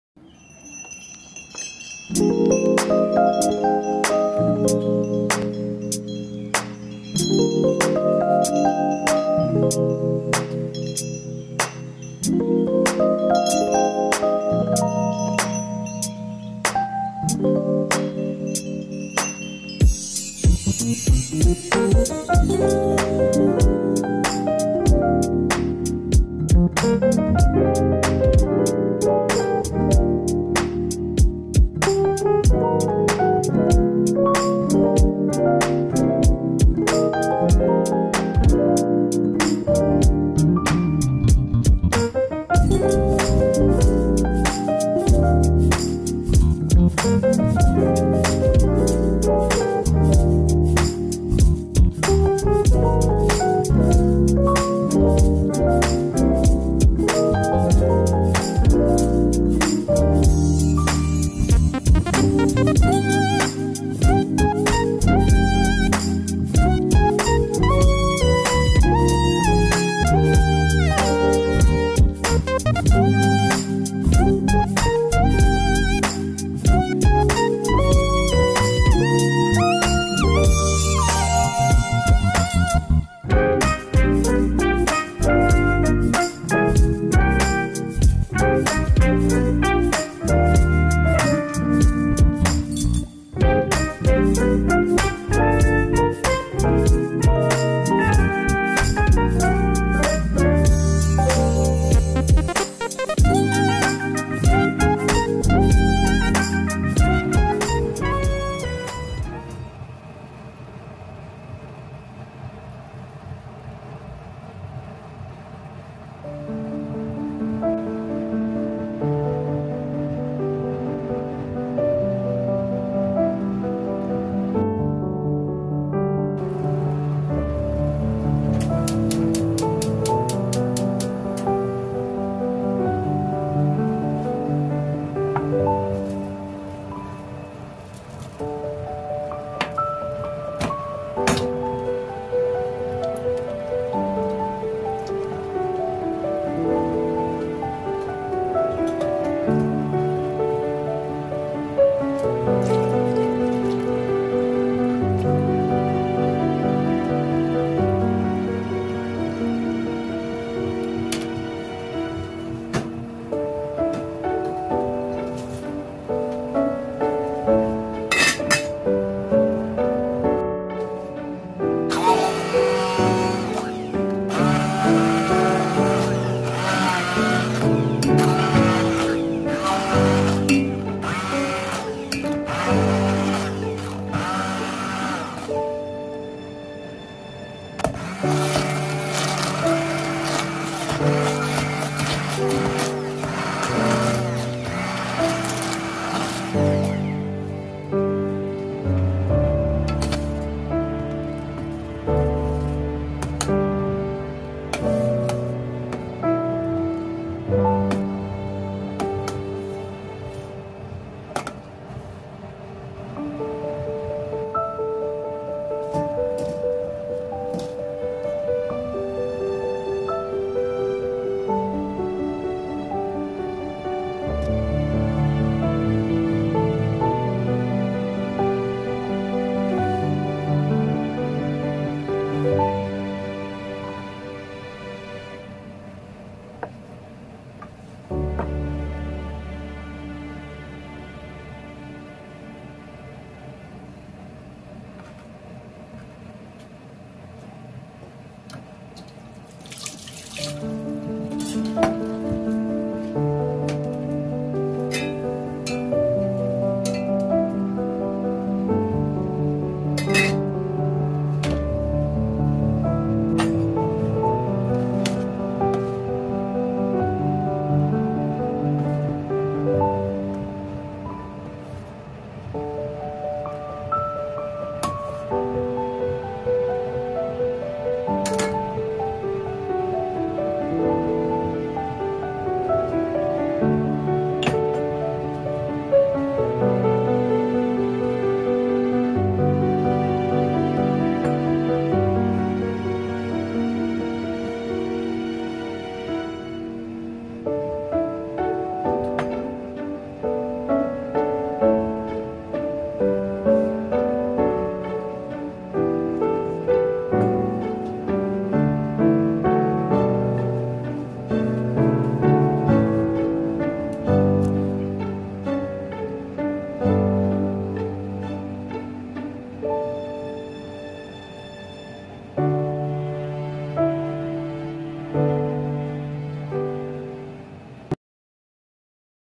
【簡単にんじんのポタージュ】No2
更年期を迎えた普通の主婦です。手抜きになりがちな主婦の昼食を、ほんの少しだけ大事にしようと…
今日は、簡単人参のポタージュと成城石井のクロワッサンです。
週1回〜2回…ゆっくり配信して行けたらと思います。
つたない動画ですが、温かい目で観て頂けたらと思います。つたない動画ですが、